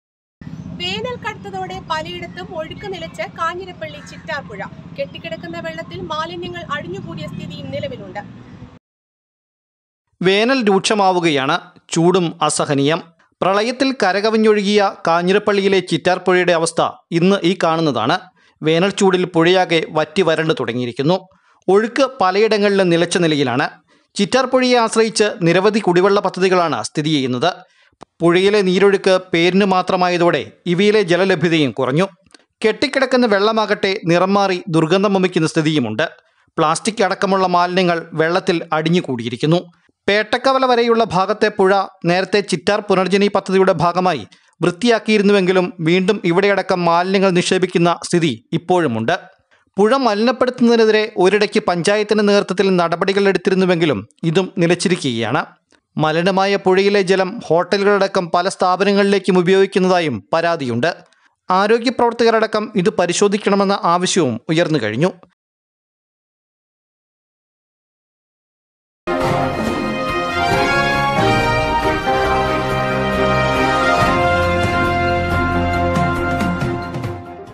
വേനൽ കടുത്തതോടെ പലയിടത്തും ഒഴുക്ക് നിലച്ച് കാഞ്ഞിരപ്പള്ളി ചിറ്റാർപ്പുഴ | Chittar Puzha
വേനൽ കടുത്തതോടെ പലയിടത്തും ഒഴുക്ക് നിലച്ച് കാഞ്ഞിരപ്പള്ളി  ചിറ്റാർപ്പുഴ. കെട്ടിക്കിടക്കുന്ന വെള്ളത്തിൽ മാലിന്യങ്ങൾ അടിഞ്ഞ് കൂടിയ സ്ഥിതിയുമുണ്ട് .